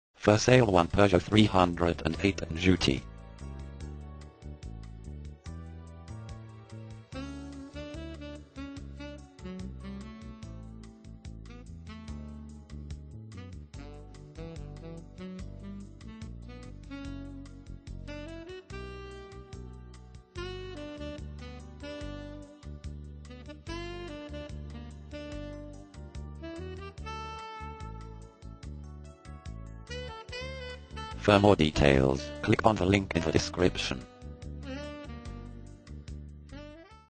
Sale one Peugeot 308 Anjoutey Territoire-de-Belfort
For sale at  Anjoutey  Used car Peugeot 308 of 30400 Km.
used motors Autoenvideo : Sale one Peugeot 308  Anjoutey  Territoire-de-Belfort